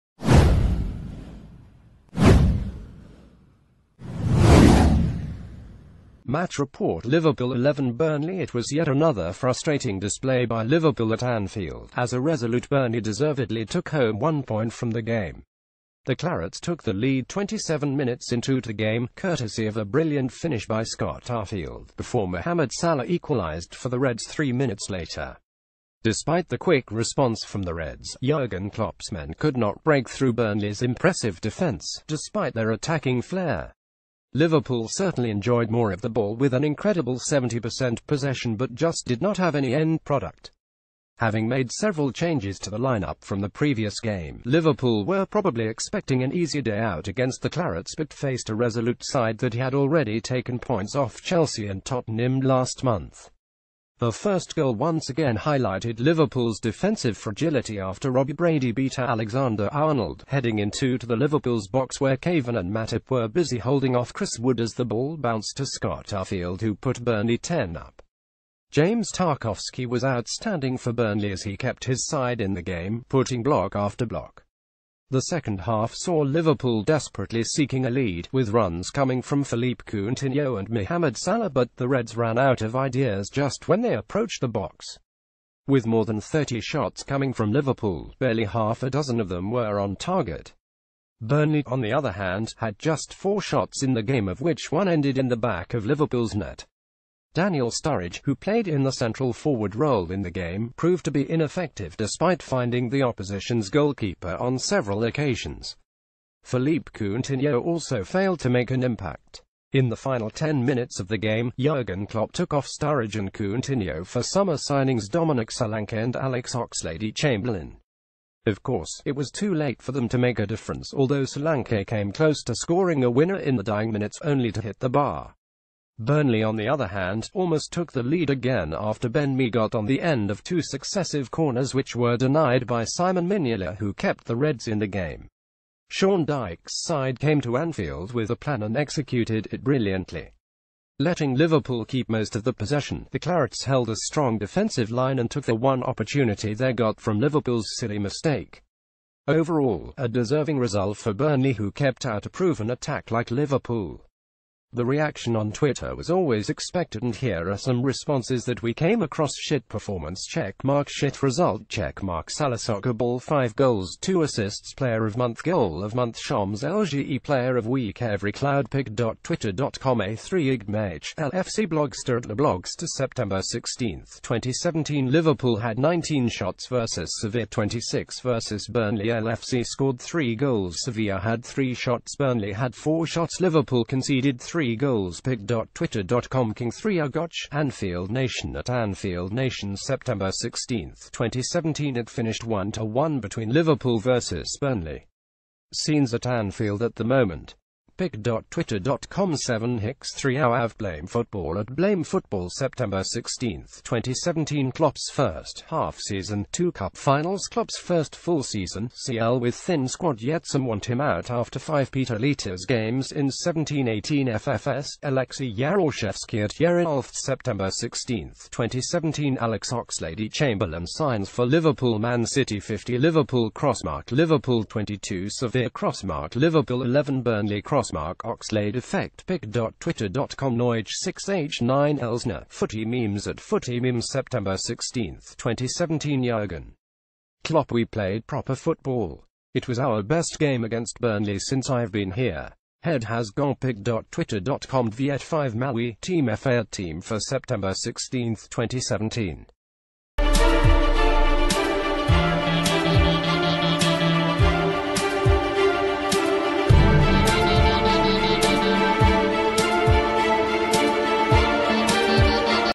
Breaking News | ‘oxlade effect’ – fans react on twitter after liverpool’s frustrating draw vs burnl
‘oxlade effect’ – fans react on twitter after liverpool’s frustrating draw vs burnley + match report

Match Report Liverpool 11 Burnley It was yet another frustrating display by Liverpool at Anfield, as...